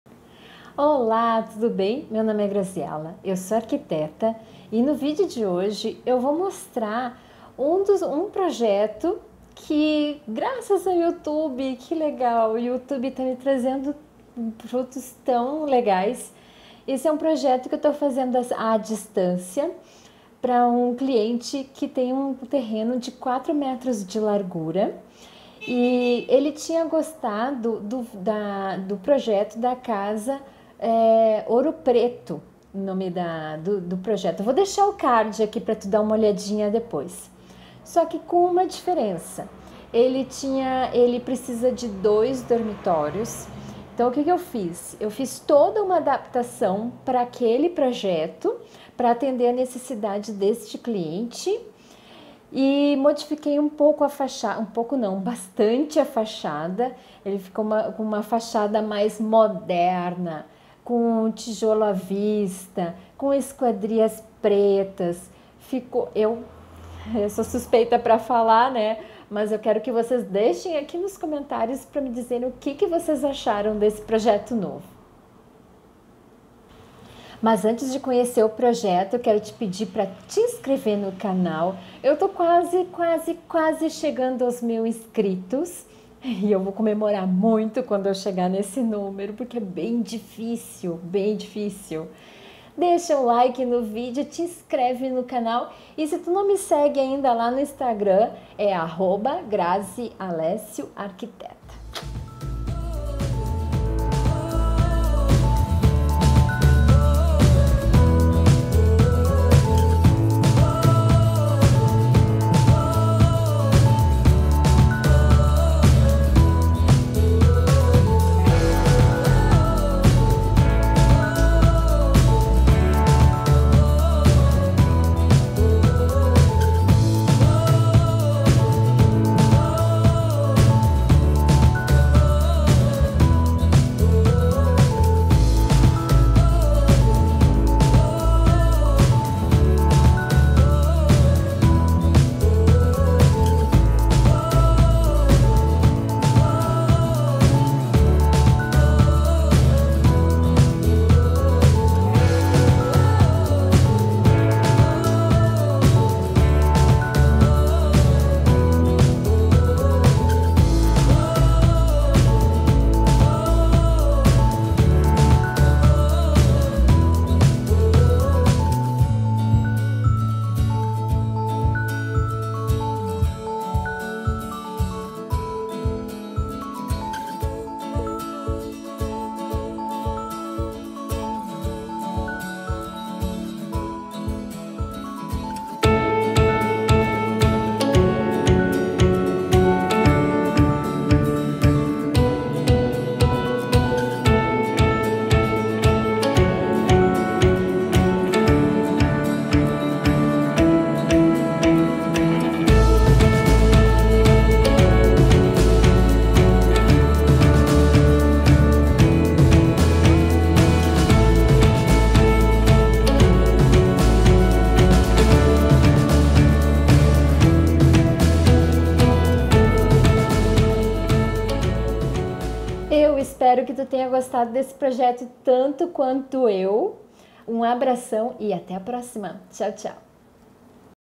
Casa Goiabeira - 2 suítes - 4m de largura
Uma casa de 143,60m2 com 2 suítes. A suíte de fundos possui sacada e a suíte da frente conta com closet e um amplo terraço. Comprimento total da casa: 17,95m2